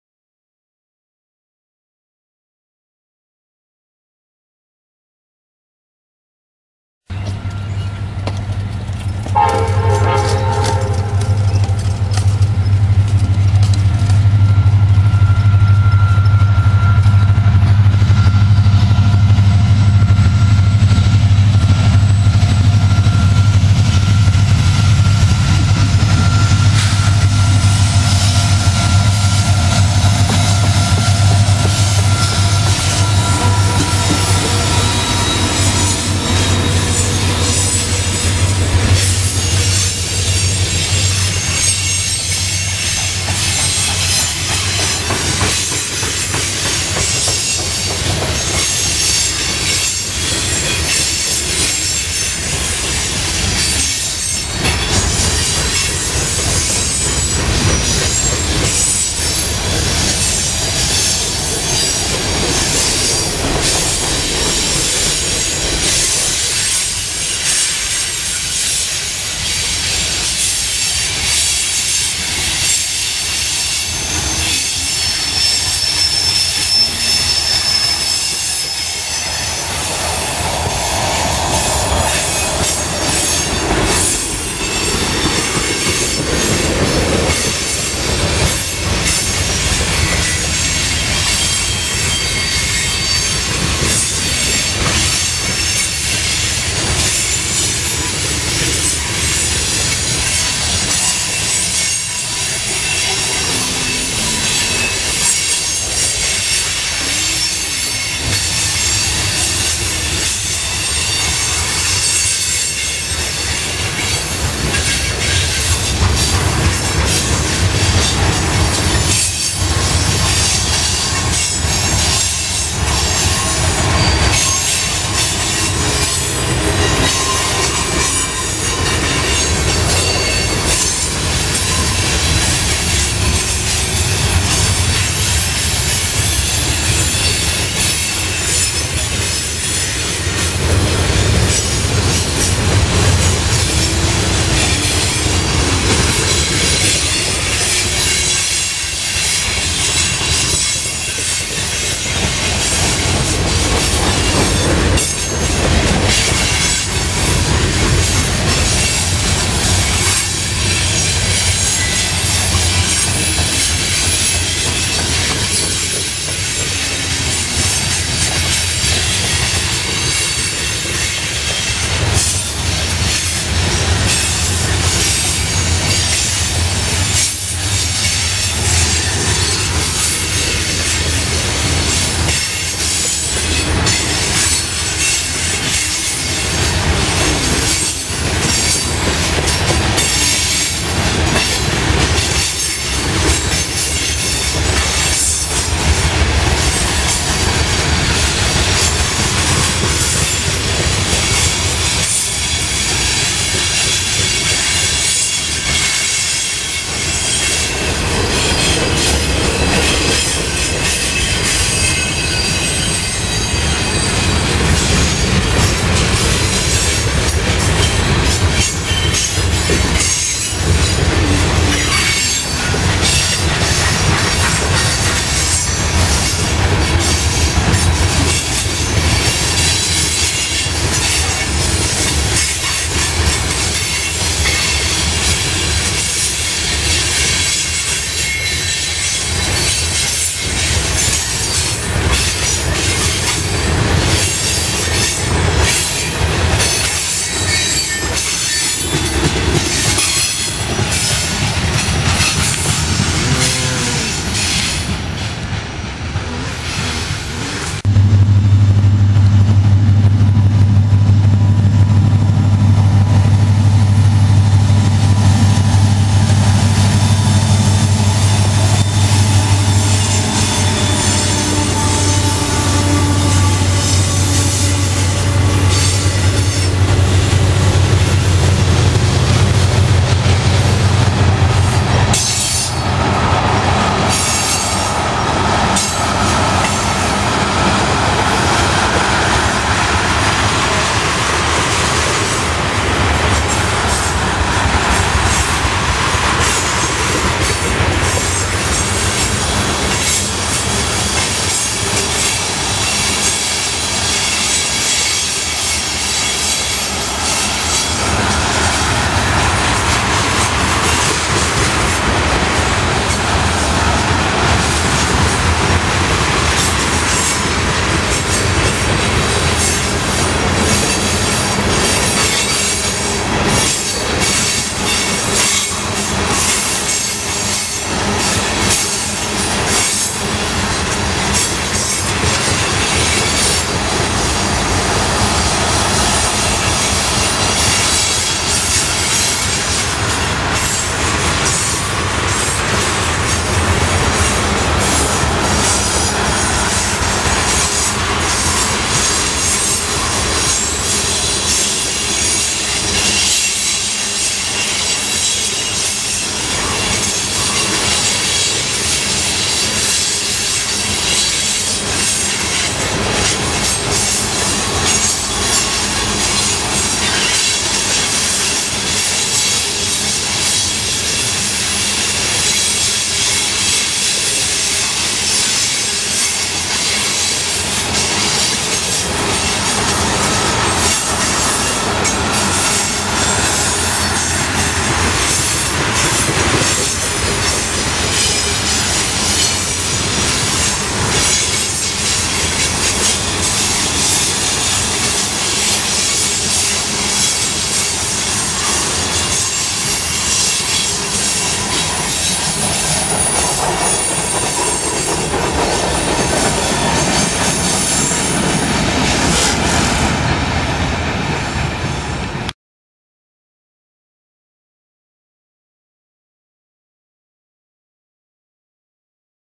Railfanning Norfolk Southern On the last day of school
This was on the last day of school for Kiski Area and I had to go cut grass but i was more than happy with what I caught to start off summer vacation.  "Second train is the best one of the night"